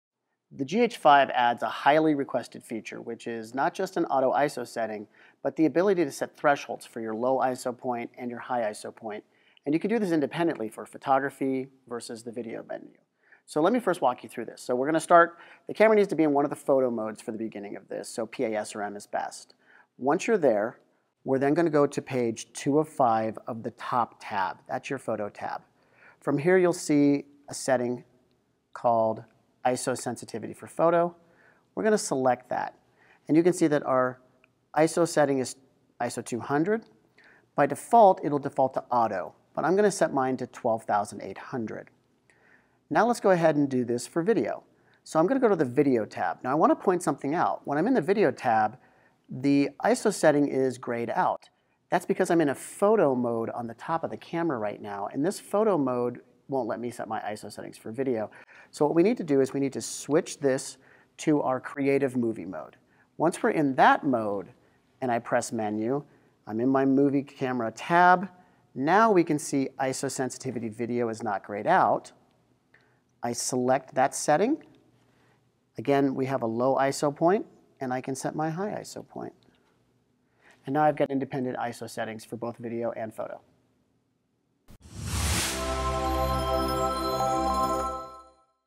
GH5 Auto ISO Thresholds
Panasonic's Tech Team Put features to the test with a quick explanation to how you set up your new GH5!